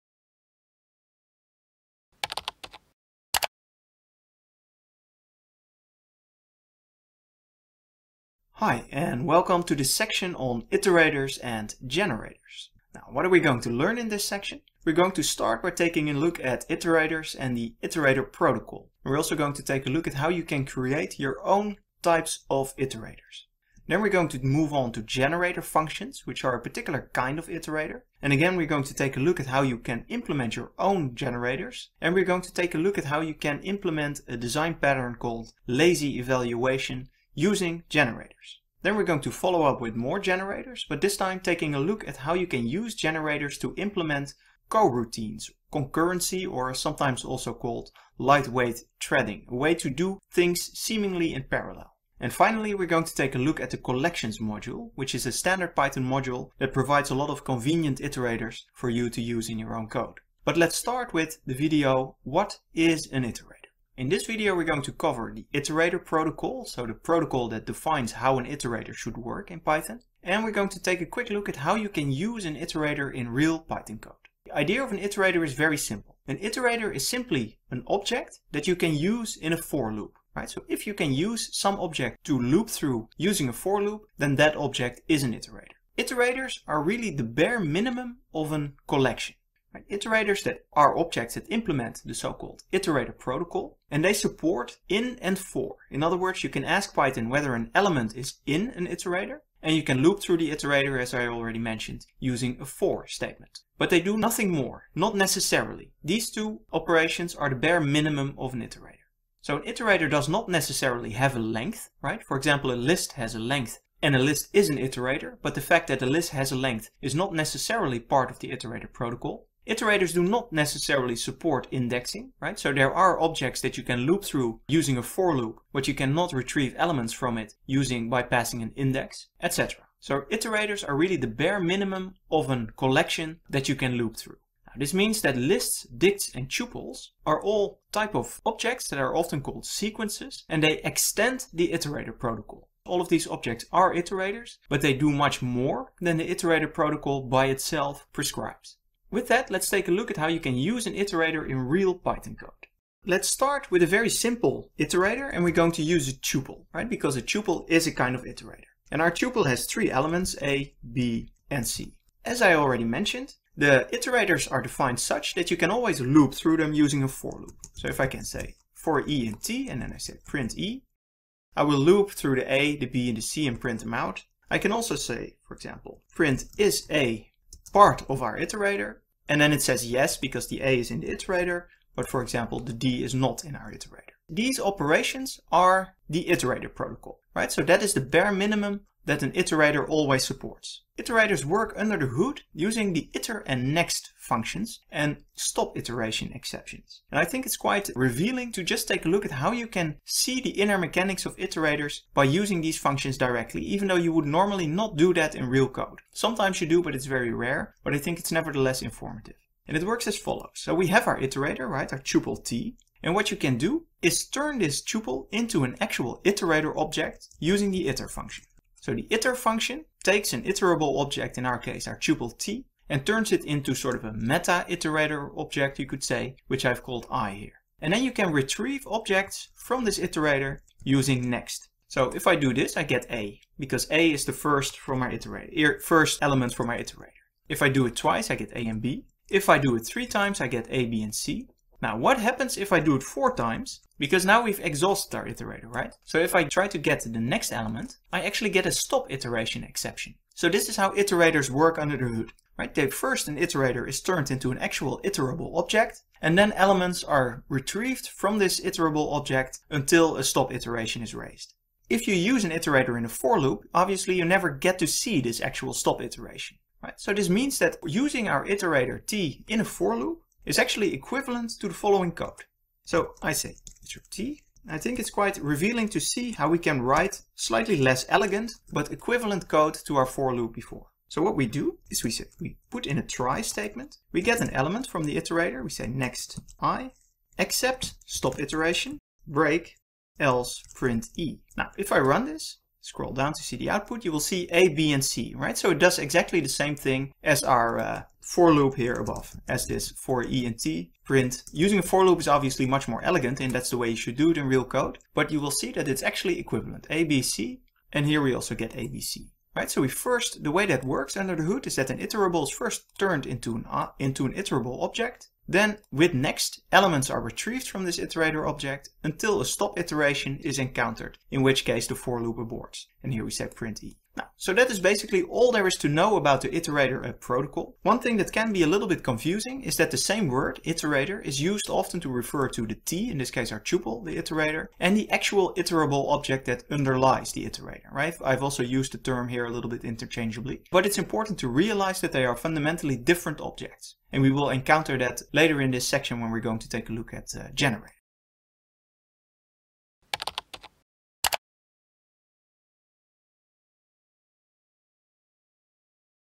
Iterators in Functional Programming with Python : What Is an Iterator? | packtpub.com
In this video, we consider theiterator protocol, which formally defines iterator objects, and describes the (minimum) functionality that iterators provide.
• Show that iterators always support for and in
• Consider iter(), next(), and StopIteration
• How to use iterators in real Python code